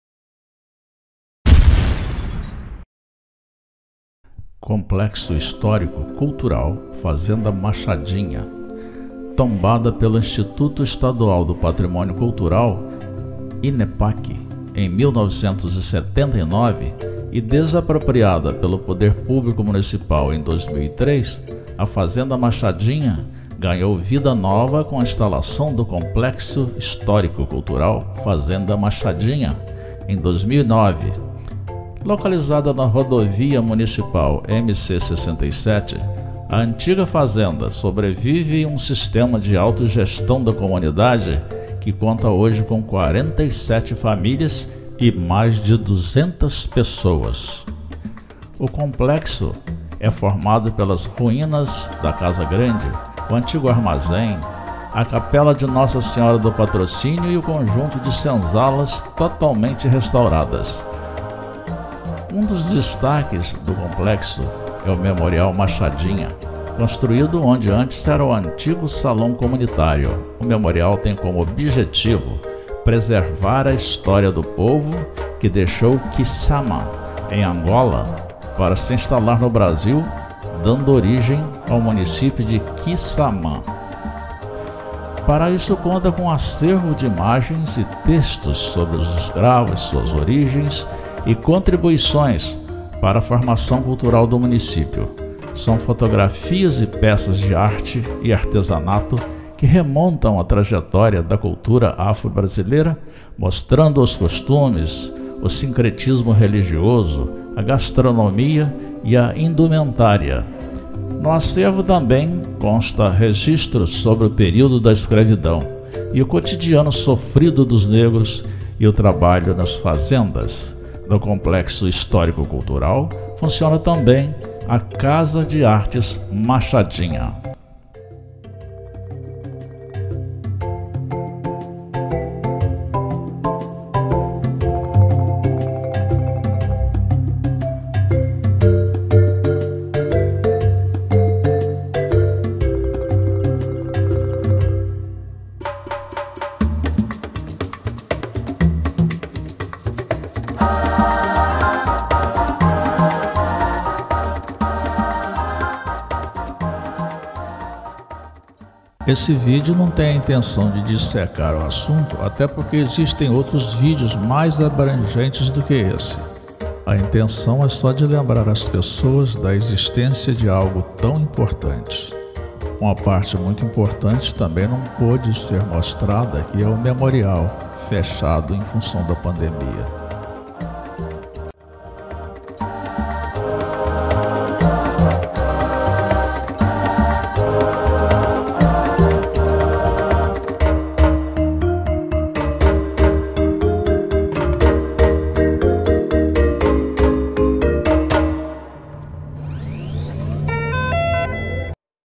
A FAZENDA MACHADINHA E SUA HISTÓRIA
Situada no município de Quissamã, no estado do Rio de janeiro, a Fazenda Machadinha é uma testemunha da época da escravidão.